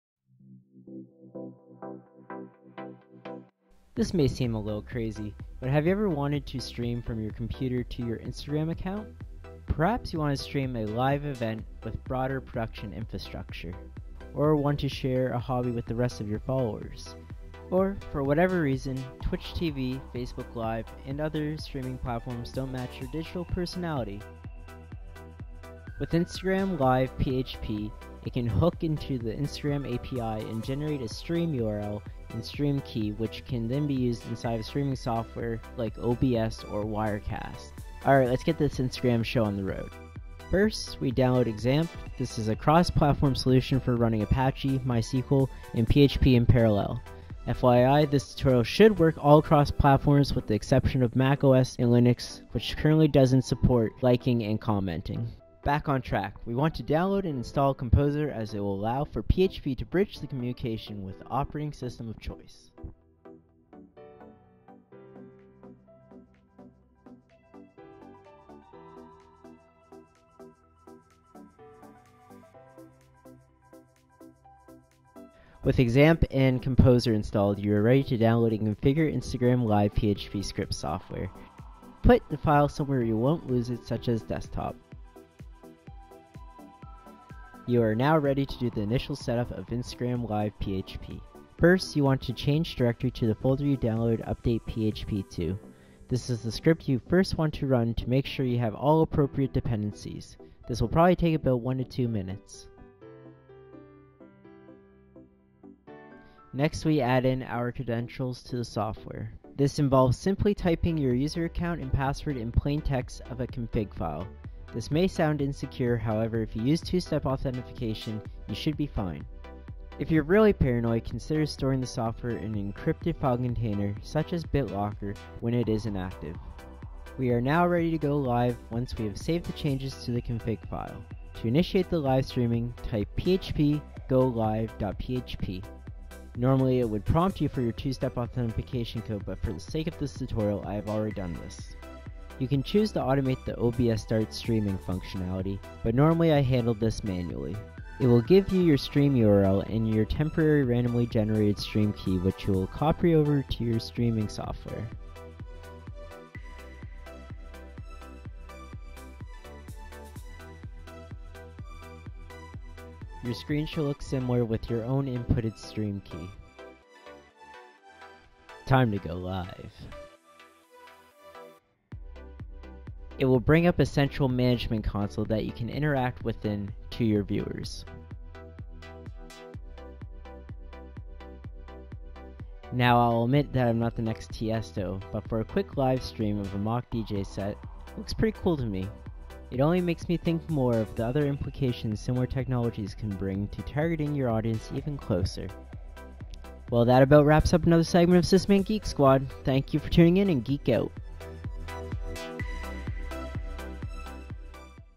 SysManGeekSquad - Streaming to Instagram using InstagramLive-PHP and OBS
EDIT (November 2022) - Now officially supported by Instagram!

EDIT (January 2020) - Instagram Live-PHP has been issued a cease and desist notice by Instagram. as an alternative :)

Quick Install Instructions
 Part 1 - Getting InstagramLive-PHP ready
 • Install PHP, of course...
 • Install Composer
 • Goto the most release release
 • Download the update.php file and place it in its own folder
 • Run the script with php update.php and let it install the script
 • Edit the USERNAME and PASSWORD inside of the config.php file to your Instagram username/password.
 • Run the goLive.php script. (php goLive.php)

 Part 2 - OBS Setup
 • Set your OBS canvas size to 720x1280. This can be done by going to Settings-Video and editing Base Canvas Resolution to "720x1280".
 • Go to the "Stream" section of your OBS Settings
 • Set "Stream Type" to "Custom Streaming Server"
 • Set the "URL" field to the stream url you got from the script
 • Set the "Stream key" field to the stream key you got from the script
 • Make Sure "Use Authentication" is unchecked and press "OK"
 • Start Streaming in OBS
To stop streaming, run the "stop" command in your terminal and then press "Stop Streaming" in OBS

Script:
This may seem a little crazy, but have you ever wanted to stream from your computer to your Instagram account?

Perhaps you want to stream a live event with broad production infrastructure, or you want to share a hobby with the rest of your followers. Or for whatever reason, Twitch.tv, Facebook Live, and other streaming platforms don't match your digital personality.

With InstagramLive-PHP, it can hook into the Instagram API and generate a Stream URL and Stream Key which can then be used inside streaming software like OBS or Wirecast.

Alright lets get this Instagram show on the road. First we download XAMPP this is a cross-platform solution for running apache, mysql, and php in parallel. FYI - this tutorial should work all across platforms - with the exception of macOS and Linux which doesn't currently doesn't support liking and commenting.

Back on track - we want to download and install composer as it will allow for php to bridge the communication with the operating system of choice.

With XAMPP and Composer installed, you are ready to download and configure InstagramLive-PHP script software. Put the file somewhere you won't lose it, such as Desktop.

You are now be ready to do the initial setup of InstagramLive-PHP. First you want to change directory to the folder you downloaded update.php to. This is the script you first want to run, to make sure you have all appropriate dependencies. This will probably take about 1-2mins.

Next we add in our credentials to the software. This involves simply typing your user account and password in plain-text of a config file. This may sound insecure, however if you use two-step authentication, you should be fine. If you're really paranoid, consider storing the software in an encrypted file container - such as BitLocker - when it is inactive.

We are now ready to go live once we have saved the changes to the config file. To initiate live streaming, type php goLive.php

Normally it would prompt you for your Two-step authentication code, but for the sake of this tutorial, I've already done this.
You can choose to do automate the OBS "Start Streaming" functionality, but normally I handle this manually.

It will give you your stream URL and your temporary randomly-generated stream key which you will copy over to your streaming software.

Your screen should look similar - with your own inputted stream key.

Time to go live.

It will bring up a central management console, that you can interact within - to your viewers.

Now I'll admit I'm not the next Tiesto, but for a quick live stream of a mock DJ set, looks pretty cool to me. It only makes me think more of the other implications similar technologies can bring to targeting your audience even closer.

Well that about wraps up another segment of SysManGeekSquad! Thank you for tuning in and geek-out.